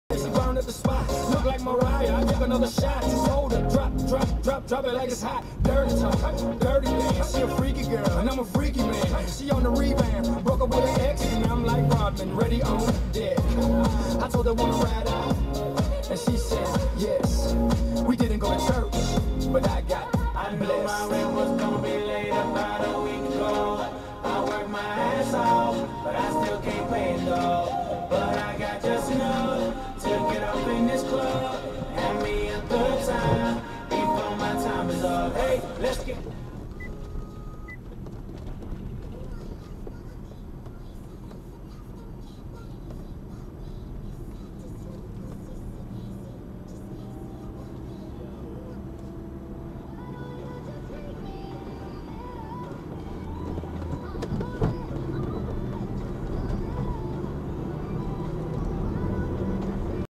ESSA 040 É UMA VERGONHA, ASFALTO LIXO, VIATURAS IDEM, PARDAL A DAR COM PAU
Não há razoabilidade para esses imbecis ficarem nessa marcha lenta. Não tem NADA QUE JUSTIFIQUE, NADA, NADA, NADA.